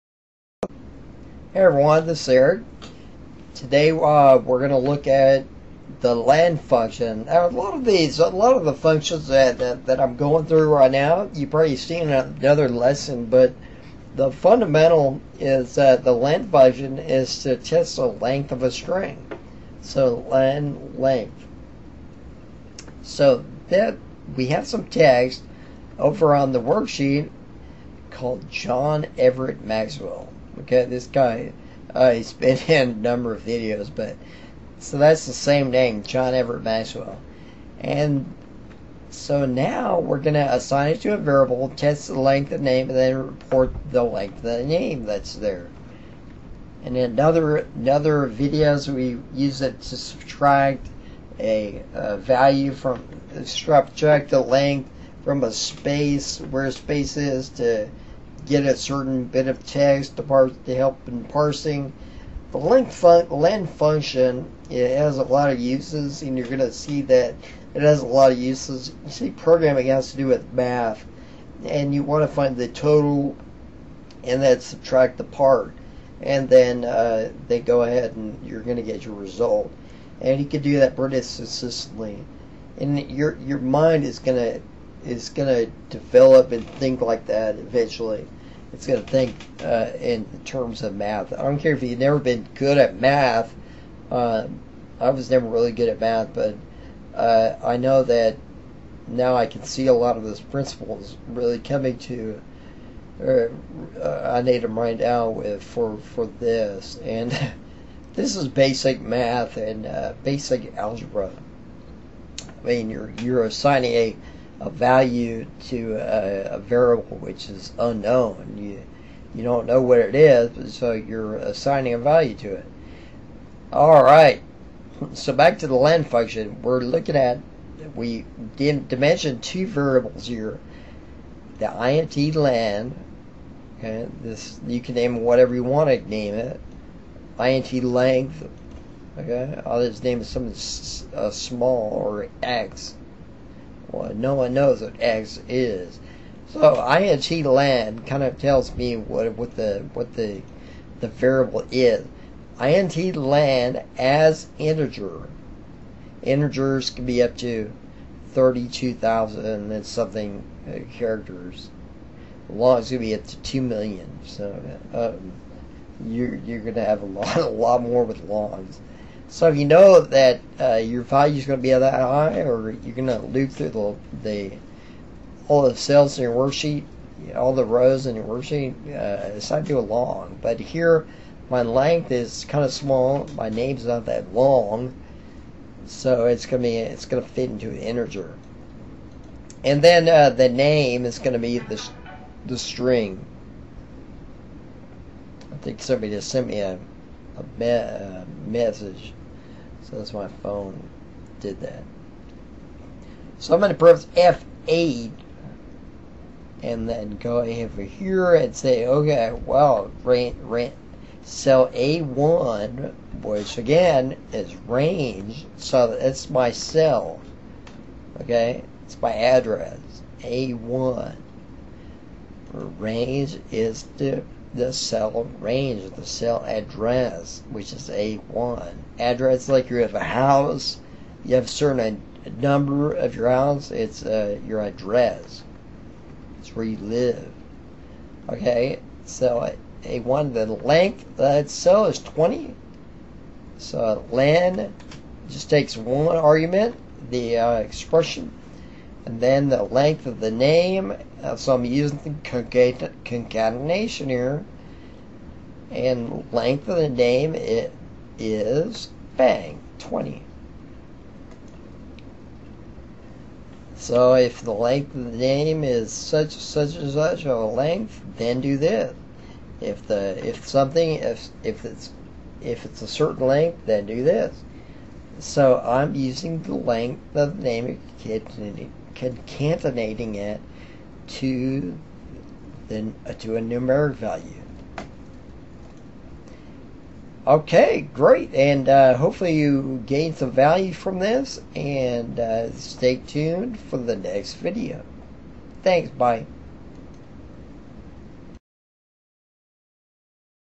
Test Length Of Text String - VBA Len Function
Use this useful VBA Len Function to test the length of your text strings.

Now watch our VBA Instr Video!

Problems sorting out the VBA information you just learned?